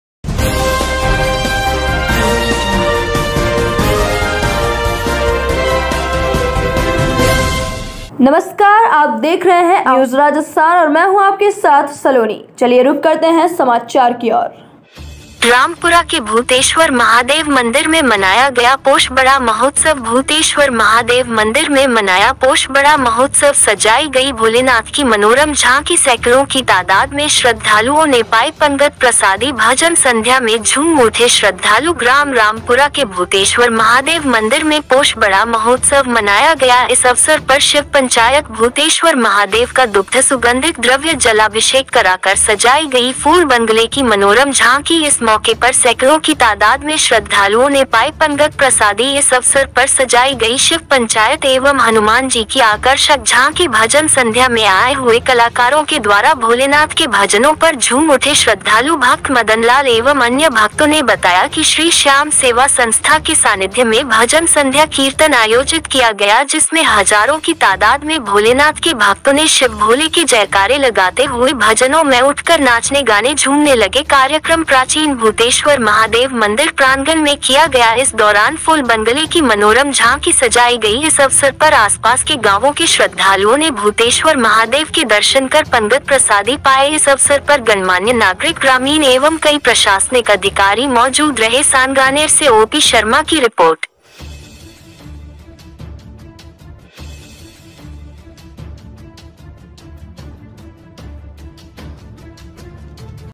महोत्सव भूतेश्वर महादेव मंदिर में मनाया पोष बड़ा महोत्सव
Awaam news Rajasthan is India,s best Hindi news channel.

लोकेशन सांगानेर. रिपोर्टर ओपी शर्मा रामपुरा के भूतेश्वर महादेव मंदिर में मनाया गया पोष बड़ा महोत्सव भूतेश्वर महादेव मंदिर में मनाया पोष बड़ा महोत्सव सजाई गई भोलेनाथ की मनोरम झांकी सैकड़ों की तादाद में श्रद्धालुओं ने पाई पंगत प्रसादी भजन संध्या में झूम उठे श्रद्धालु ग्राम रामपुरा के भूतेश्वर महादेव मंदिर में पोष बड़ा महोत्सव मनाया गया इस अवसर पर शिव पंचायत भूतेश्वर महादेव का दुग्ध सुगंधित द्रव्य जलाभिषेक कराकर सजाई गई फूल बंगले की मनोरम झांकी इस मौके पर सैकड़ों की तादाद में श्रद्धालुओं ने पाई पंगत प्रसादी इस अवसर पर सजाई गई शिव पंचायत एवं हनुमान जी की आकर्षक झांकी भजन संध्या में आए हुए कलाकारों के द्वारा भोलेनाथ के भजनों पर झूम उठे श्रद्धालु भक्त मदनलाल एवं अन्य भक्तों ने बताया कि श्री श्याम सेवा संस्था के सानिध्य में भजन संध्या कीर्तन आयोजित किया गया जिसमें हजारों की तादाद में भोलेनाथ के भक्तों ने शिव भोले के जयकारे लगाते हुए भजनों मैं उठकर नाचने गाने झूमने लगे कार्यक्रम प्राचीन भूतेश्वर महादेव मंदिर प्रांगण में किया गया इस दौरान फुल बंगले की मनोरम झांकी सजाई गई इस अवसर पर आसपास के गांवों के श्रद्धालुओं ने भूतेश्वर महादेव के दर्शन कर पंगत प्रसादी पाई इस अवसर पर गणमान्य नागरिक ग्रामीण एवं कई प्रशासनिक अधिकारी मौजूद रहे सांगानेर से ओपी शर्मा की रिपोर्ट